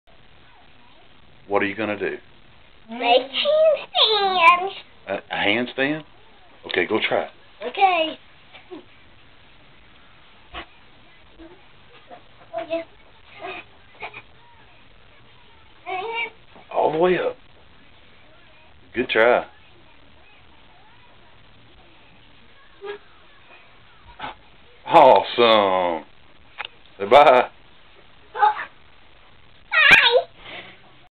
Four year old - starter gymnastics at home #1
Our son messing around and surprised me with his new found skill. lol